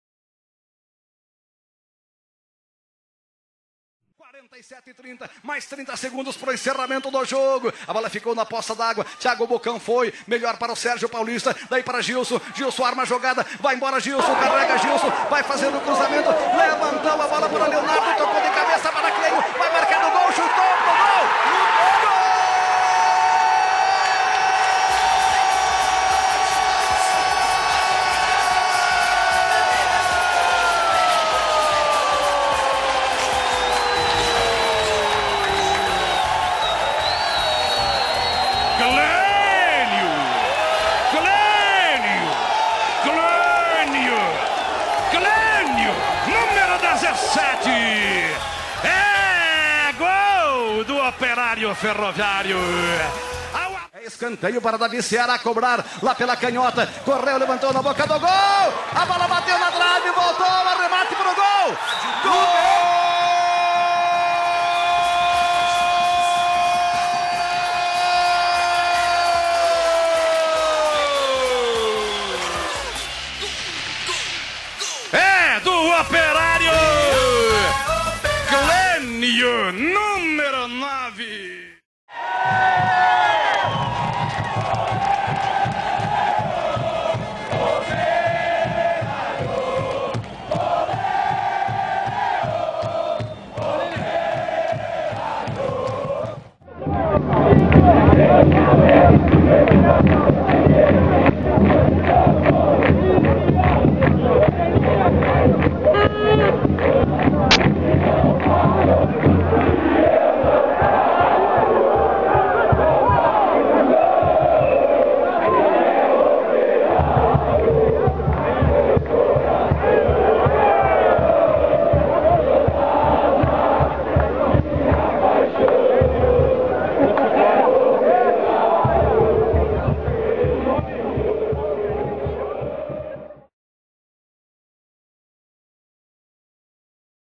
Compareça ao Germano Kruger
Torcedor operariano, como todos nós queriamos, finalmente o Operário volta a disputar o campeonato brasileiro.
Cabe agora, nós torcedores, fazermos a nossa parte, lotarmos o estádio para empurrar o fantasma rumo as vitórias.
E o próximo jogo é final de semana, contra o Joinville.
Compareça ao Germano Kruger, vista preto e branco, apoie e cante o jogo inteiro.

Torcedor de verdade é aquele que vai ao estádio .